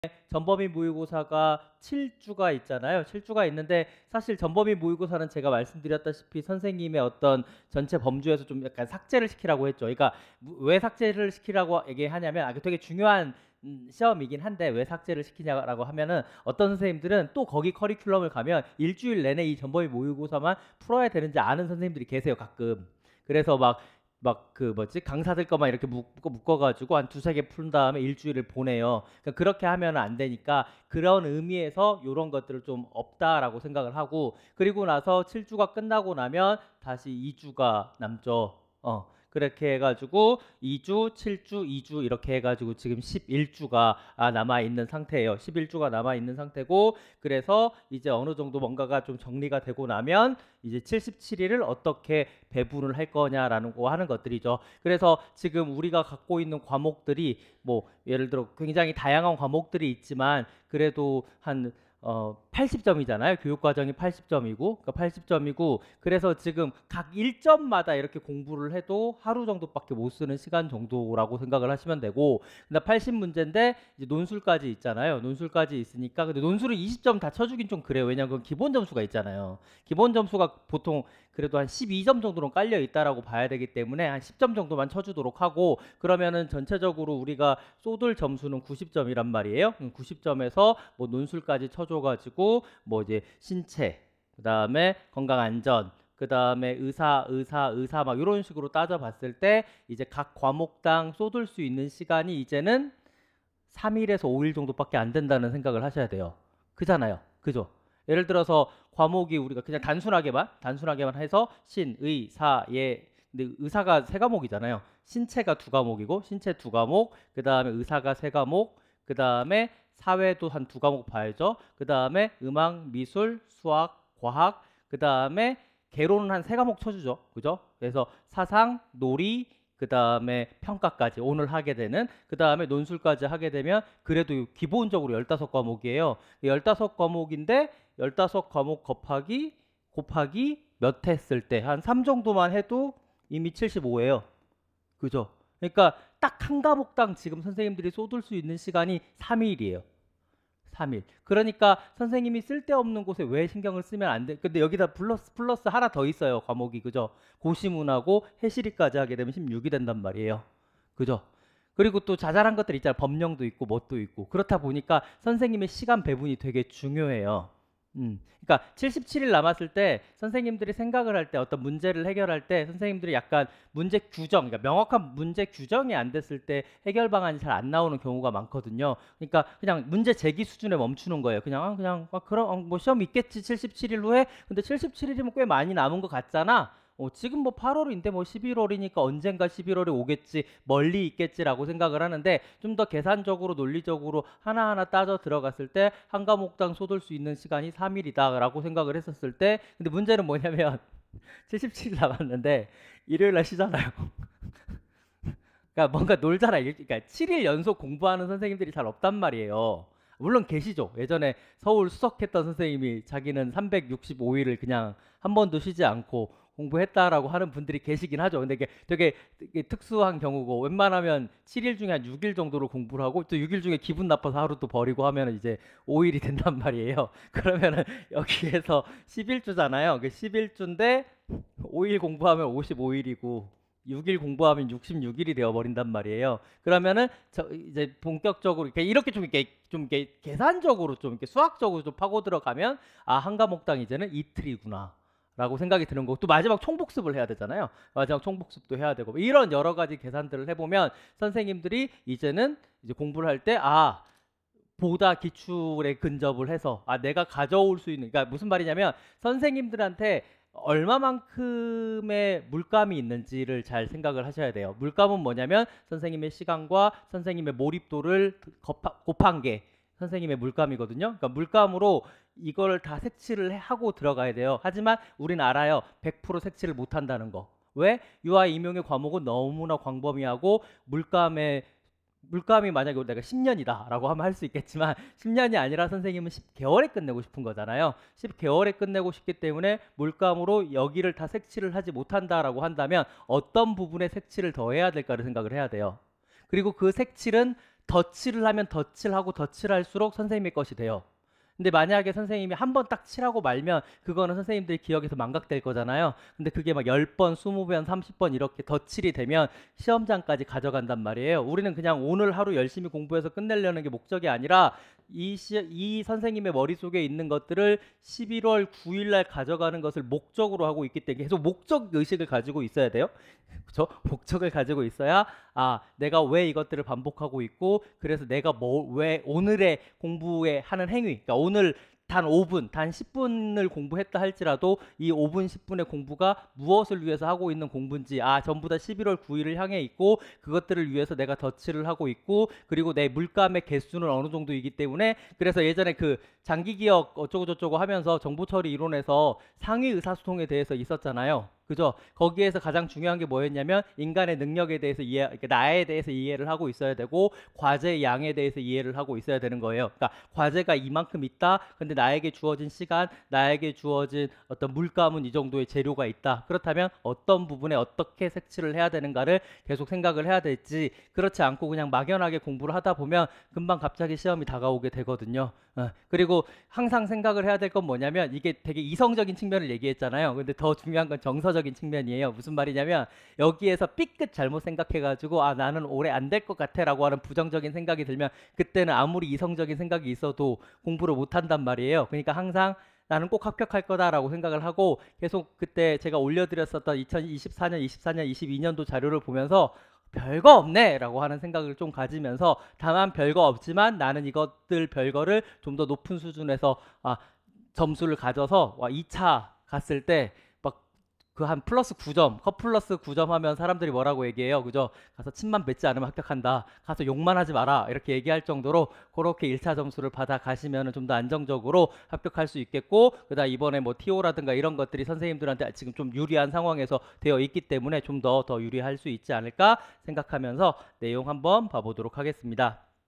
[유아 이동건] 전범위 모의고사 개강 임박! 77일을 어떻게? - 유아임용, 유아교육과
9~10월 전범위 모의고사 개강 임박!
종강 후의 시간, 개강 후 추석 기간
어떻게 활용할 것인가?